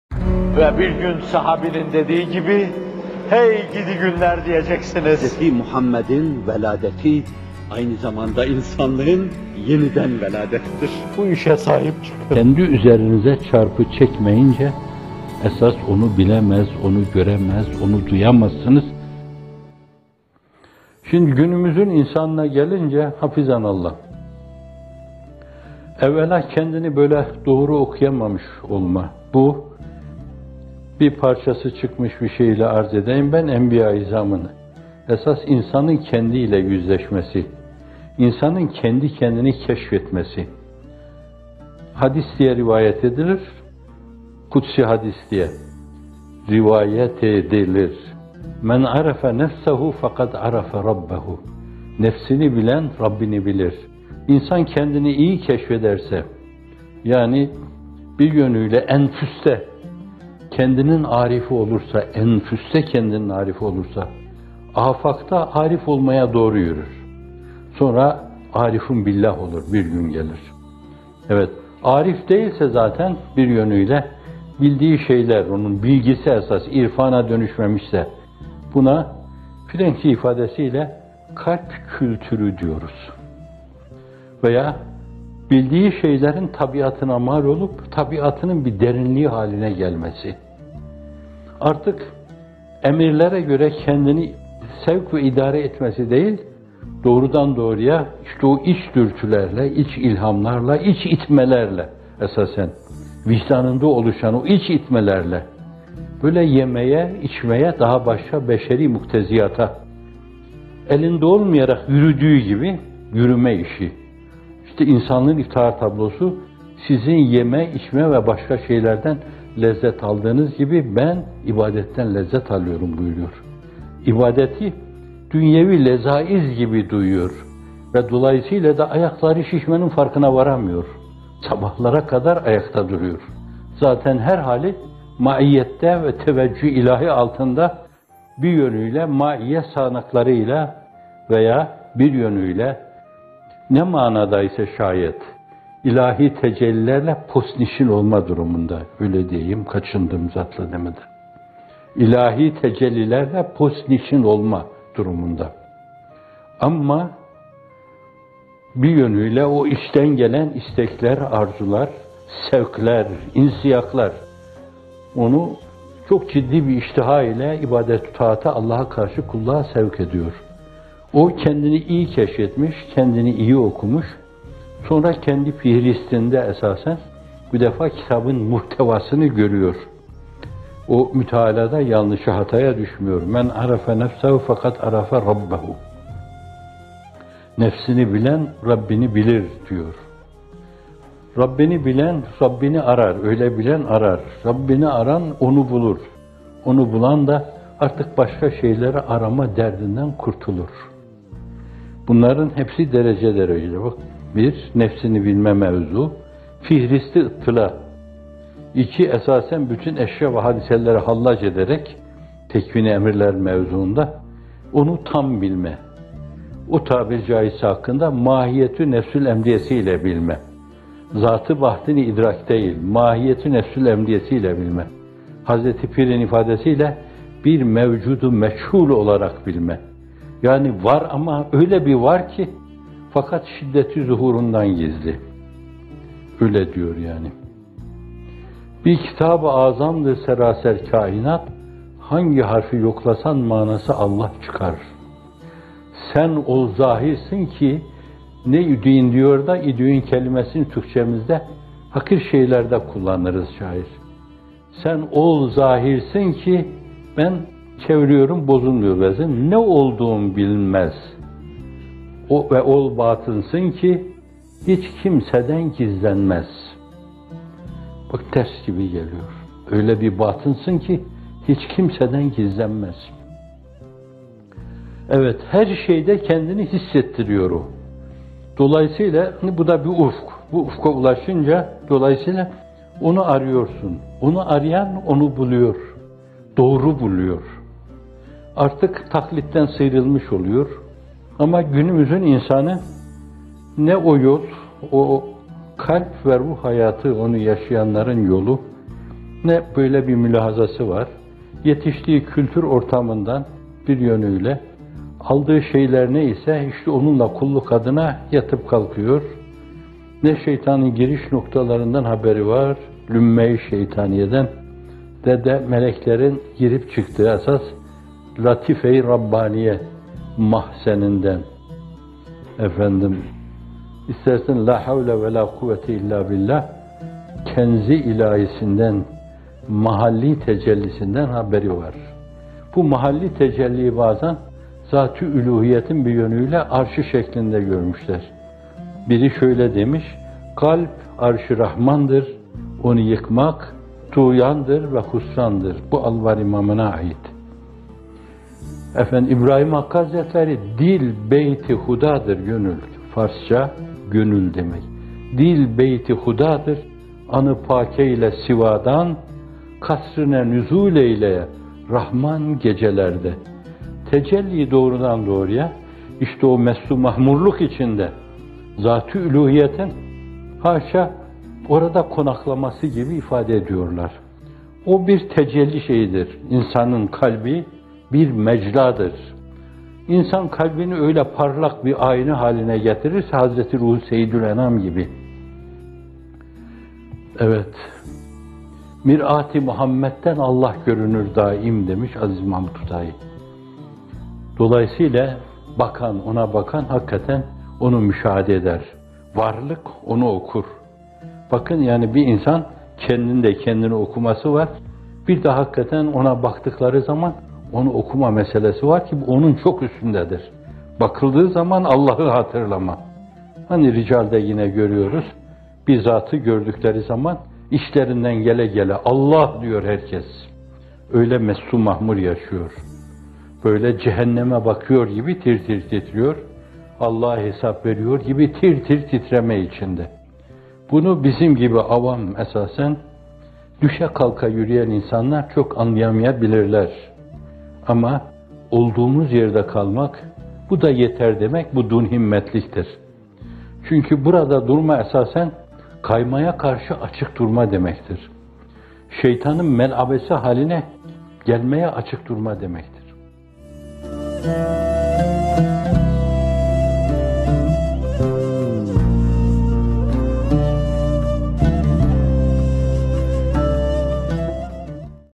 Nefsini Bilen, Rabbini Bilir! | Mizan | M. Fethullah Gülen Hocaefendi
Bu video 24/12/2017 tarihinde yayınlanan "KALBİN İKİ MERKEZİ VE ŞEYTÂNÎ OKLAR" isimli bamtelinden alınmıştır. Tamamı burada
“Nefsini bilen, Rabbini bilir!”

Şimdi günümüzün insanına gelince: Hafizanallah! Evvelâ kendini doğru okuyamamış olma söz konusu. Bunu, Çağlayan dergisinde bir bölümü çıkmış olan yazı dizisinin konusuyla arz edeyim: Enbiyâ-ı ızâmın rehberliğinde insanın kendi ile yüzleşmesi, insanın kendi kendini keşfetmesi. Hadis diye rivayet edilir, kudsî hadis diye ri-vâ-yet e-di-lir: مَنْ عَرَفَ نَفْسَهُ فَقَدْ عَرَفَ رَبَّهُ “Nefsini bilen, Rabbini bilir!” İnsan, kendini iyi keşfederse, yani bir yönüyle enfüste kendinin ârifi olursa, âfâkta da ârif olmaya doğru yürür; sonra bir gün gelir “ârif billah” olur. Evet, “ârif” değilse zaten, bir yönüyle, bildiği şeyler, onun bilgisi esas irfana dönüşmemiş demektir. Bu marifete, Frenkçe ifadesiyle “kalb kültürü” diyoruz veya bildiği şeylerin tabiatına mal olup, tabiatının bir derinliği haline gelmesi. Artık emirlere göre kendini sevk ve idare etmesi değil, doğrudan doğruya o iç dürtüler ile, iç ilhamlar ile, iç itmeler ile, vicdanında oluşan o iç itmeler ile hareket etmesi… Yemeğe, içmeye, daha başka beşerî mukteziyata elinde olmayarak yürüdüğü gibi yürümesi…

İşte İnsanlığın İftihar Tablosu, (sallallâhu aleyhi ve sellem) “Sizin, yemeden, içmeden ve başka şeylerden lezzet aldığınız gibi, Ben, ibadetten lezzet alırım!” buyuruyor. İbadeti, dünyevî lezâiz gibi duyuyor ve dolayısıyla da ayaklarının şiştiğinin farkına varamıyor, sabahlara kadar ayakta duruyor. Zaten her hali, maiyyet ve teveccüh-i İlahî altında, maiyyet sağanakları ile veya -bir yönüyle- ne manada ise şayet, “İlahî tecelliler ile postnişin olma” durumunda… -Öyle diyeyim, kaçındım “Zat ile” demekten.- “İlahî tecelliler ile postnişin olma” durumunda. O içten gelen istekler, arzular, sevkler, insiyaklar, O’nu çok ciddî bir iştiha ile ibadet u tâate, Allah’a karşı kulluğa sevk ediyor. O (sallallâhu aleyhi ve sellem), kendini iyi keşfetmiş, kendini iyi okumuş, sonra kendi fihristinde bu defa Kitâb’ın muhtevasını görüyor. O mütalaada yanlışa/hataya düşmüyor:

Evet, مَنْ عَرَفَ نَفْسَهُ فَقَدْ عَرَفَ رَبَّهُ “Nefsini bilen, Rabbini bilir!” deniyor. Rabbini bilen, Rabbini arar; öyle bilen, arar. Rabbini arayan, O’nu bulur. O’nu bulan da artık başka şeyleri arama derdinden kurtulur. Bunların hepsi, derece derecedir. Bakın: Bir; nefsini bilme mevzuu, fihriste ıttılâ. İki; bütün eşya ve hadiseleri hallaç ederek, tekvinî emirler mevzuunda, O’nu (celle celâluhu) tam bilme. O tabir caiz ise O’nun hakkında, mâhiyet-i nefsü’l-emriyesini bilme; “Zât-ı Baht’ı idrak” değil, mâhiyet-i nefsü’l-emriyesiyle bilme. Hazreti Pîr’in ifadesiyle, “Bir Mevcûd u Meçhul” olarak bilme; yani, “Var ama öyle bir var ki, şiddet-i zuhûrundan gizli!”, öyle diyor.

“Bir Kitâb-ı âzâmdır, serâser kâinat,

Hangi harfi yoklasan, manası “Allah” çıkar.”

“Sen, Ol Zâhir’sin ki, ne idüğün bilinmez,

Ve Ol Bâtın’sın ki, hiç kimseden gizlenmez!..”

Şâir, “idüğün” diyor da, “idüğün” kelimesi Türkçemizde hakir şeylerde kullanılır; “Sen Ol Zâhir’sin ki… -Ben çeviriyorum, bozulmuyor vezin.- ne olduğun bilinmez!” Bakın, ters gibi geliyor; “Öyle bir Bâtın’sın ki, hiç kimseden gizlenmez!”.

   “Kalb, arş-ı Rahman’dır; onu yıkmak, tuğyandır ve hüsrandır!”

Bakıldığı zaman, Allah’ı hatırlama… Hani Ricâl’de yine okuyoruz; bir zatı gördükleri zaman, içinden gele gele “Allah!” diyor herkes. Öyle mest u mahmur yaşıyor. Böyle Cehennem’e bakıyor gibi, tir tir titriyor; Allah’a hesap veriyor gibi tir tir titreme içinde. Bunu bizim gibi avam, düşe-kalka yürüyen insanlar, çok anlayamayabilirler; fakat olduğumuz yerde kalmak, “Bu da yeter!” demek de dûn-himmetliktir. Çünkü burada durma -esasen- kaymaya karşı açık durma, şeytanın mel’abesi (oyuncağı) haline gelmeye açık durma demektir.

►Bizimle paylaşmak istediğiniz haberler için e-posta adresimiz 👉